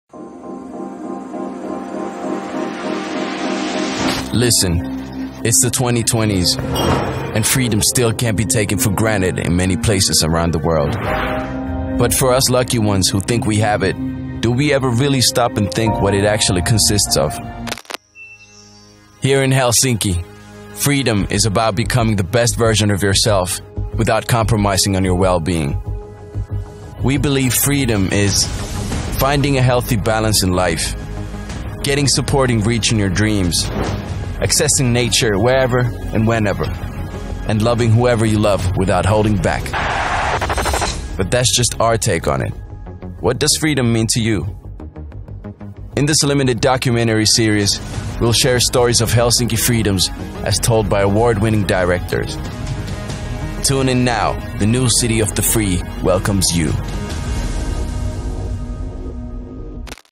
Helsinki Freedom
Video a cura di Helsinki Marketing, proiettato durante il convegno internazionale "Smart Tourism and the Global Challenge" del 26 novembre 2020